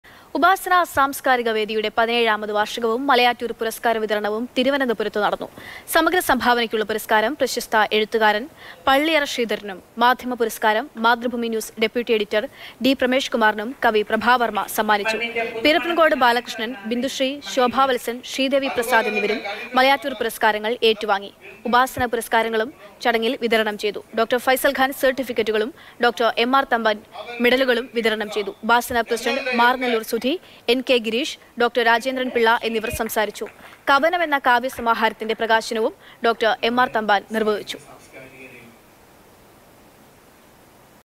മലയാറ്റൂര്‍ പുരസ്‌കാര വിതരണം; മാധ്യമ പുരസ്‌കാരം മാതൃഭൂമി ഡെപ്യൂട്ടി എഡിറ്റര്‍ ഡി പ്രമേഷ് കുമാറിന്
ഉപാസന സാംസ്‌കാരിക വേദിയുടെ 17-ാമത് വാര്‍ഷികവും മലയാറ്റൂര്‍ പുരസ്‌കാര വിതരണവും തിരുവനന്തപുരത്ത് നടന്നു.

Happy viewing!